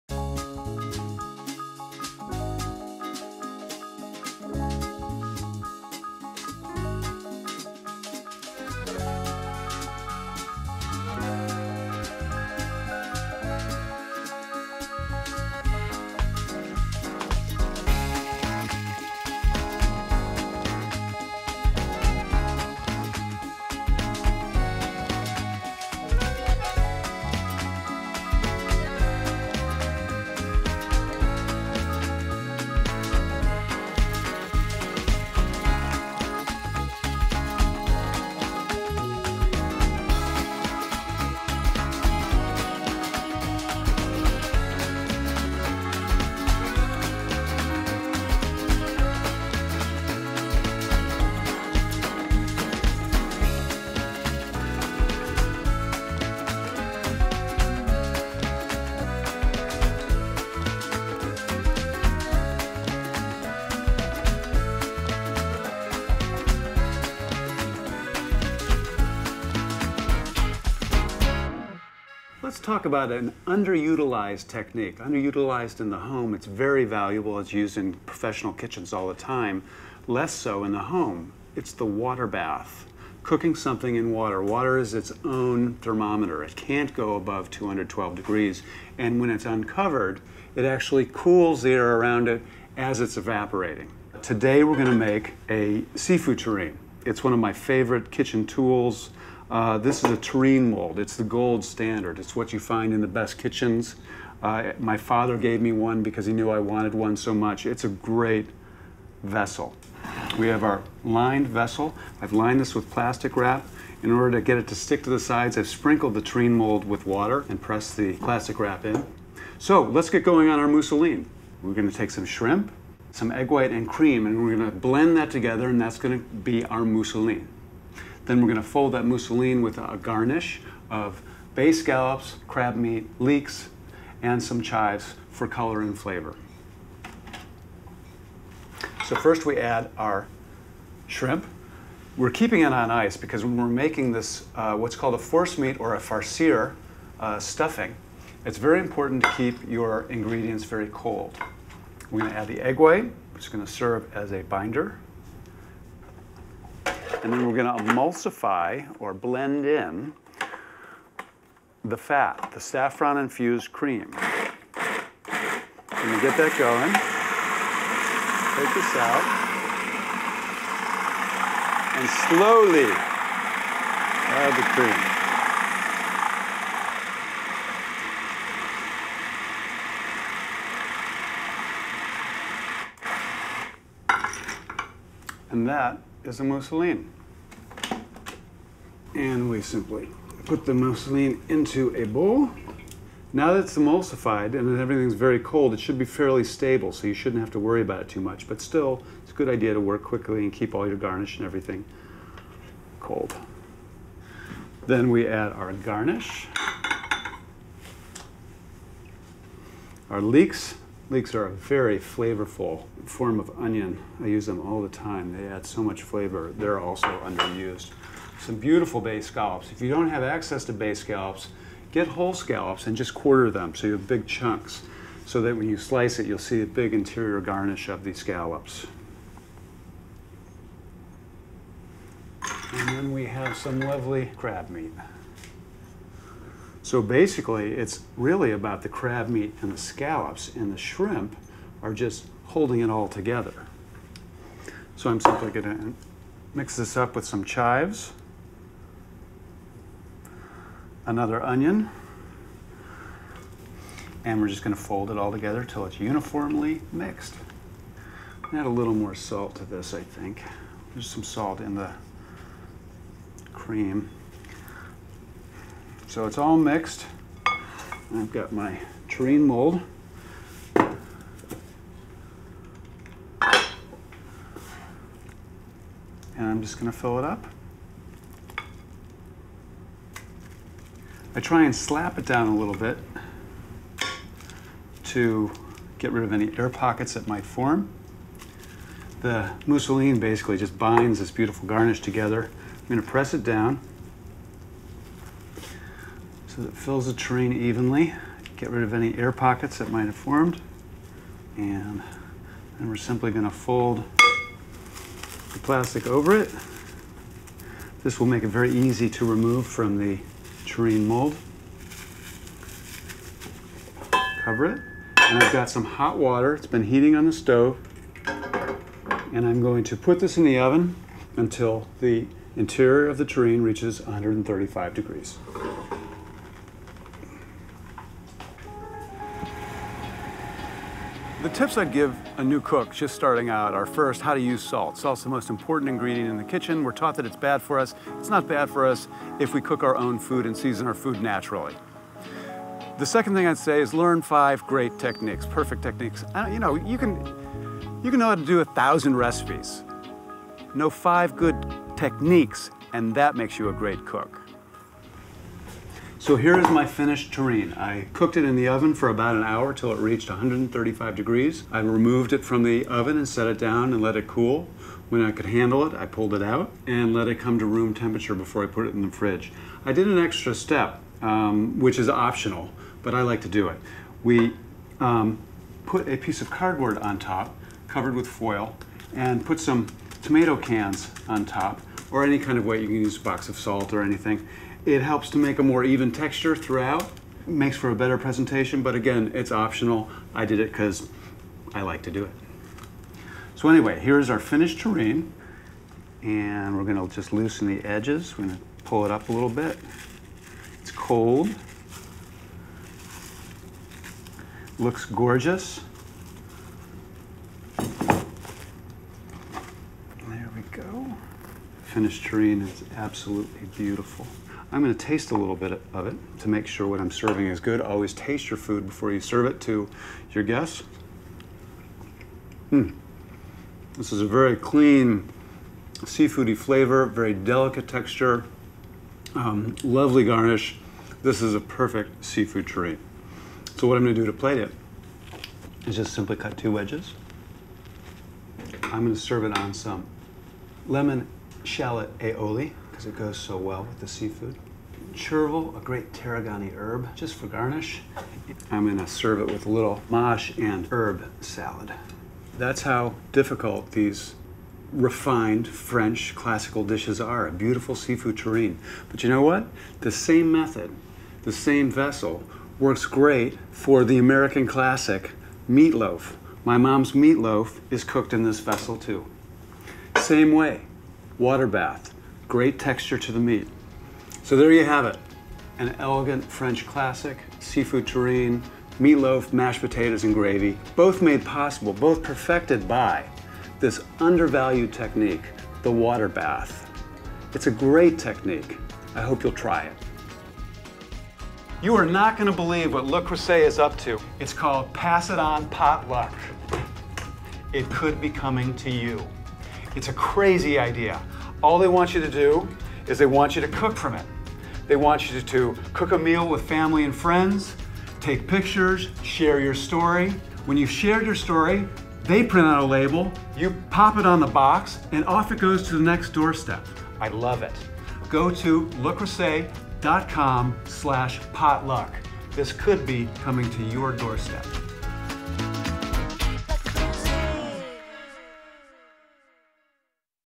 The Le Creuset Technique Series with Michael Ruhlman - Terrines
There's nothing more classically French than an elegant terrine--we've been producing the definitive cast iron molds for making them for nearly a century. In this installment Michael presents a timeless recipe for traditional seafood terrine using a raw bar worthy assortment of blended shrimp, scallops and crabmeat cooked gently in a water bath and then applies the same technique to create a new spin on the ultimate comfort food with a modern meatloaf.